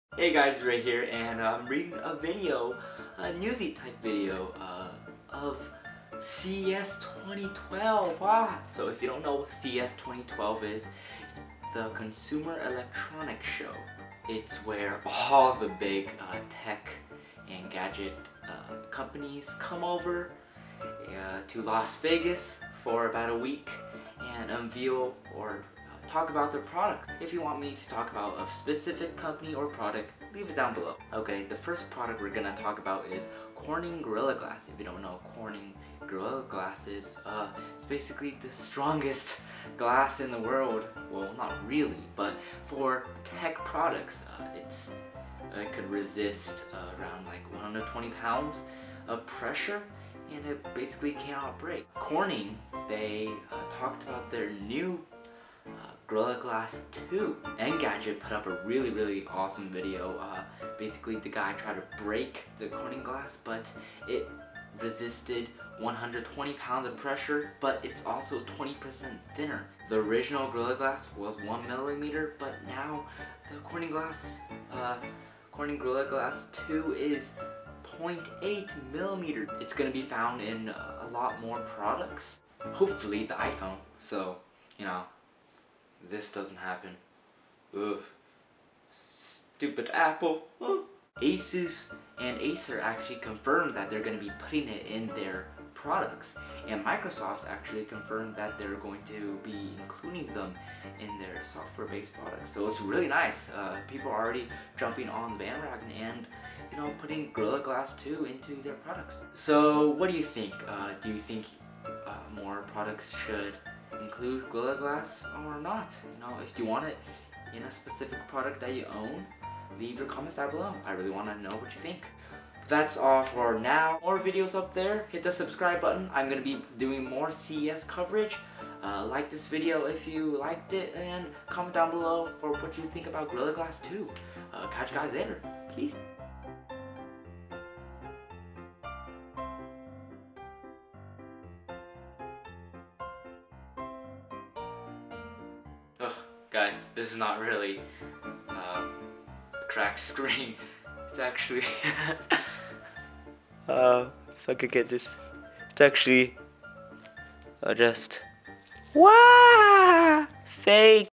First Look: Corning Gorilla Glass 2 (CES 2012)
Details of Gorilla Glass 2 by Corning that was recently unveiled at CES 2012.
Please comment to show your opinions of The Gorilla Glass 2.

**NOTE: Interactive videos/Annotations may not work with portable devices.**

Corning Gorilla Glass 2 CES 2012